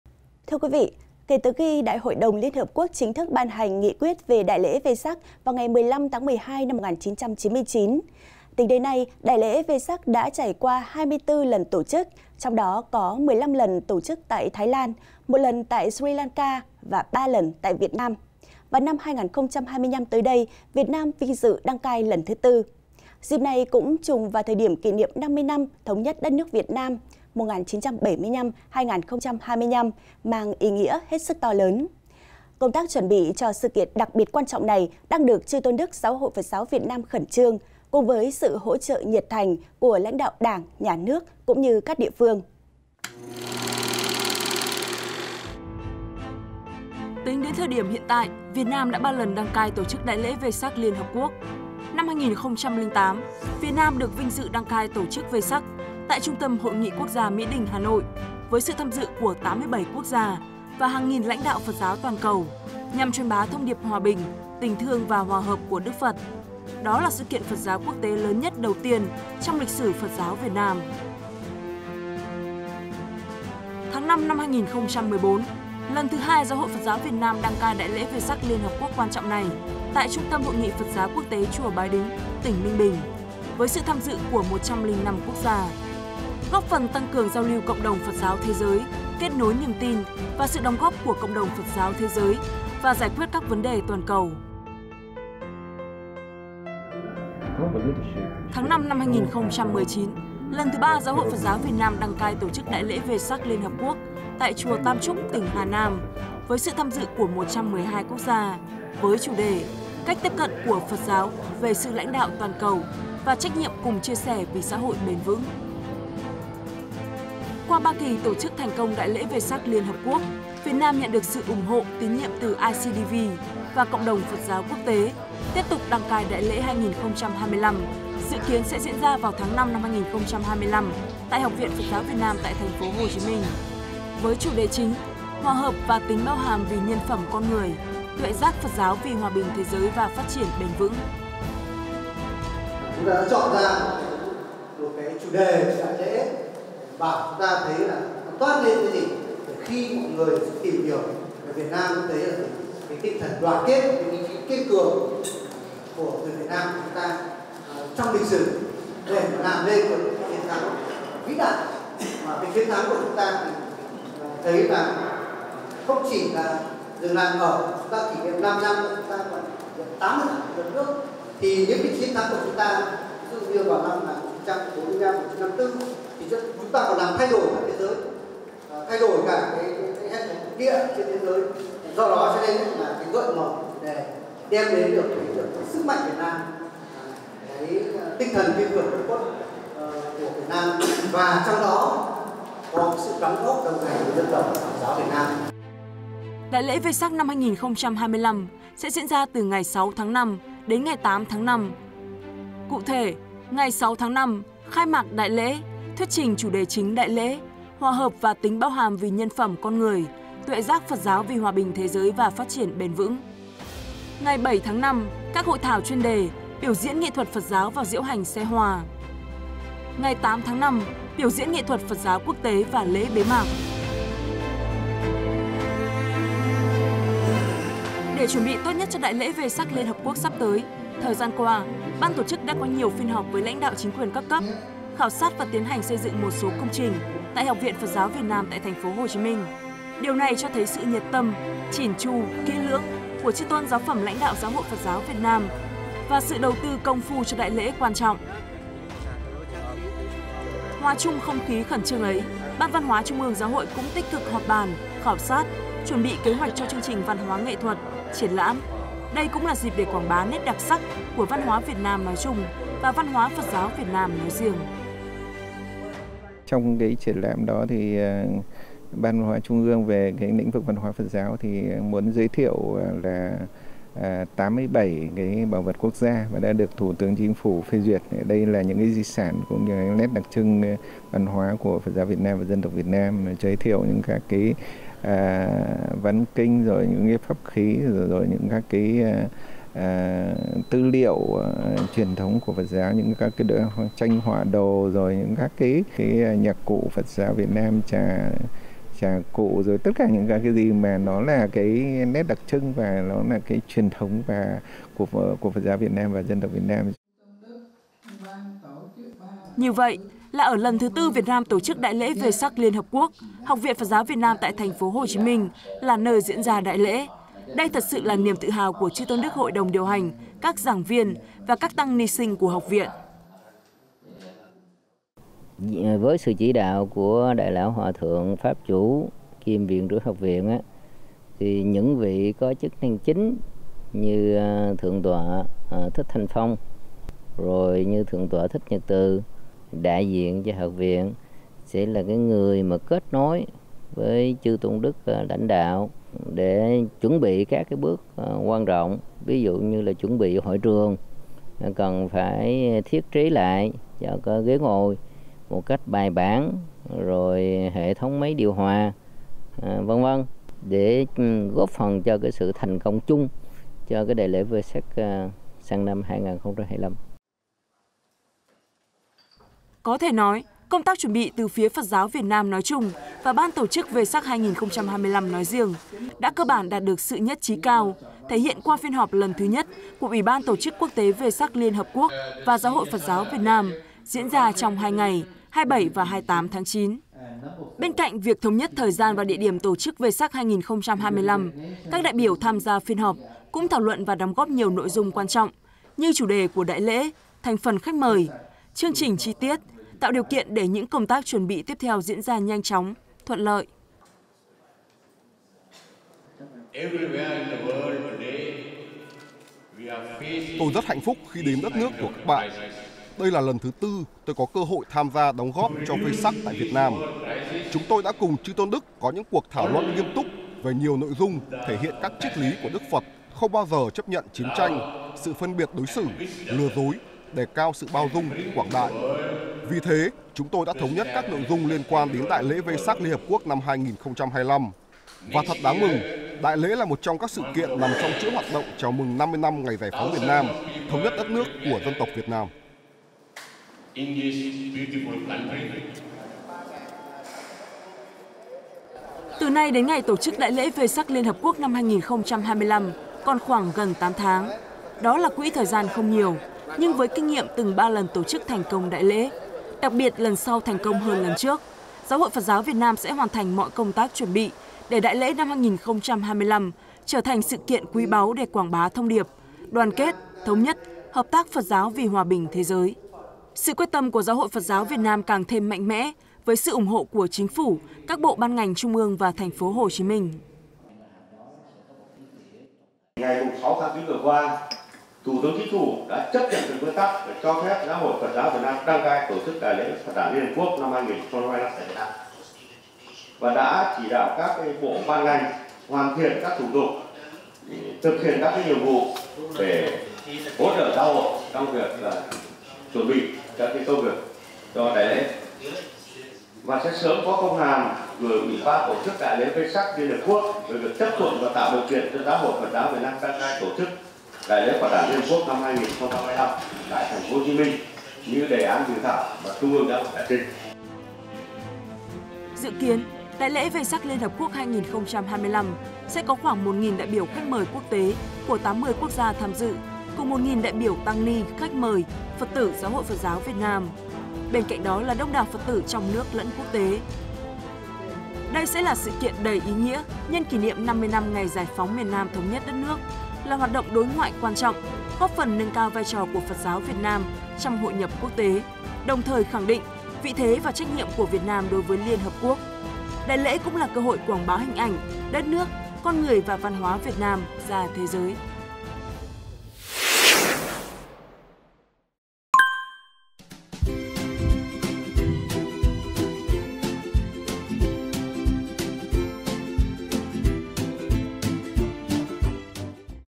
Tiêu điểm: Phật giáo Việt Nam chuẩn bị Đại lễ Vesak Liên Hợp Quốc | Bchannel
Kể từ khi Đại Hội đồng Liên Hợp Quốc chính thức ban hành Nghị quyết về Đại lễ Vesak vào ngày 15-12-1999, tính đến nay Đại lễ Vesak đã trải qua 24 lần tổ chức, trong đó có 15 lần tổ chức tại Thái Lan, 1 lần tại Sri Lanka và 3 lần tại Việt Nam. Và năm 2025 tới đây, Việt Nam vinh dự đăng cai lần thứ 4. Dịp này cũng trùng vào thời điểm kỷ niệm 50 năm thống nhất đất nước Việt Nam (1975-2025), mang ý nghĩa hết sức to lớn. Công tác chuẩn bị cho sự kiện đặc biệt quan trọng này đang được chư tôn đức GHPGVN khẩn trương; cùng với sự hỗ trợ nhiệt thành của lãnh đạo Đảng, Nhà nước cũng như các địa phương.